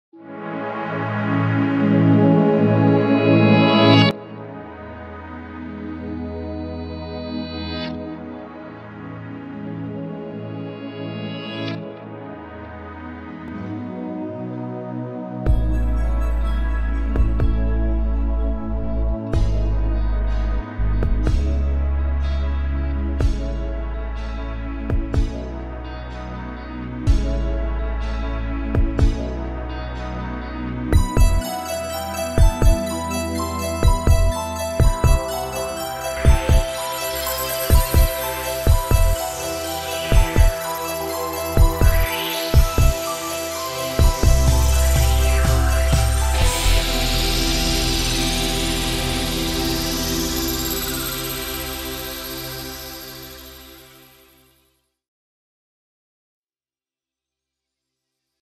Eastern Fantasy:A Metaverse Science Fiction Fest 2.0
The promotional video for a science fiction festival in metaverse, hosted by the Penglai Science Fiction Institute and many major organizations joined. There will be a special room dedicated to the late maser Brian Aldiss.The Chengdu 2023 Worldcon committee has representatives to speak as well.